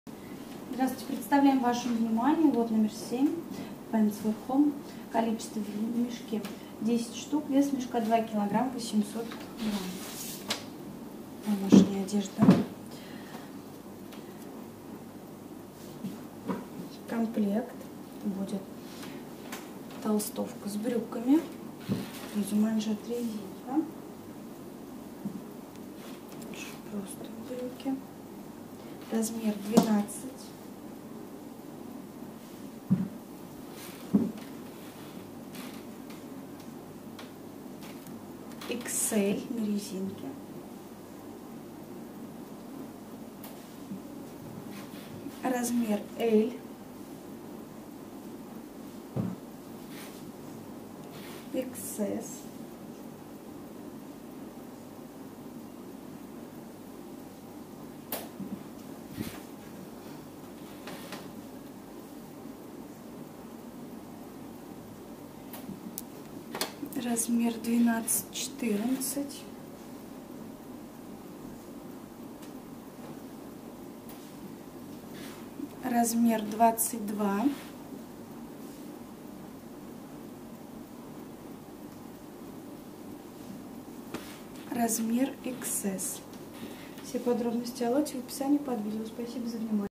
Pants for home 7, Цена кг : 8.42 евро ,секонд хенд одежда оптом
Pants for home (лот № 7)
Демисезон
Страна: Германия
Цена за кг.: 8.42 евро
Вес мешка, кг.: 2.8
Цена за мешок: 23.57 евро